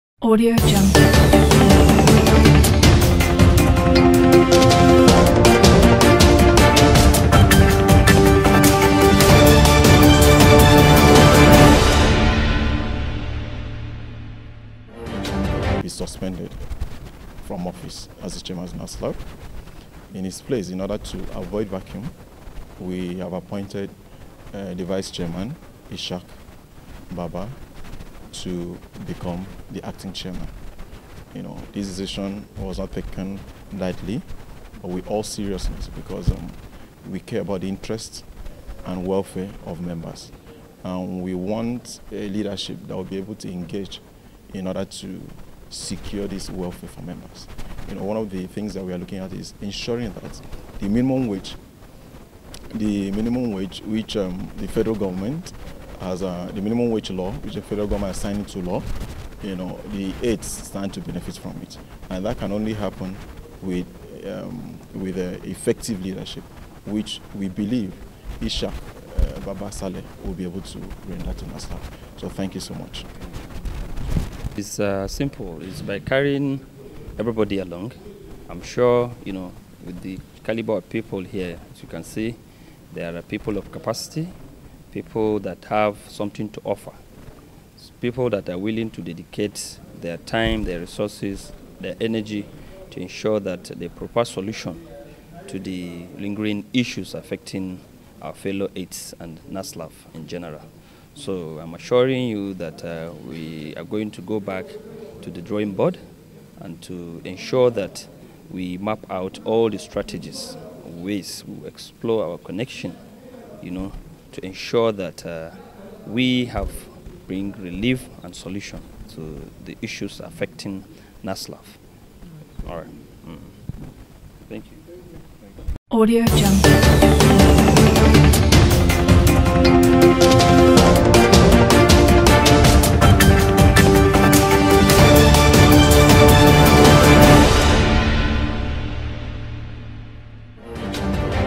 Uncertainty As Legislative Aides Suspend Chairman, Nwala From Office
There is uncertainty among legislative aides as the Central Working Committee (CMC) of the National Assembly Legislative Aides Forum (NASSLAF) suspended its chairman, Emeka Nwala for alleged violation of its constitution.

A statement issued by the Public Relations Officer of NASSLAF, Toyota Jimmy in a statement in Abuja said, “Today, Monday, 28 October, 2024, the Central Working Committee (CWC), the body charged with the responsibility to, among others, elect and take disciplinary action against officers and members of the Forum, suspended Mr Emeka Nwala from the office of the Chairman of the Forum.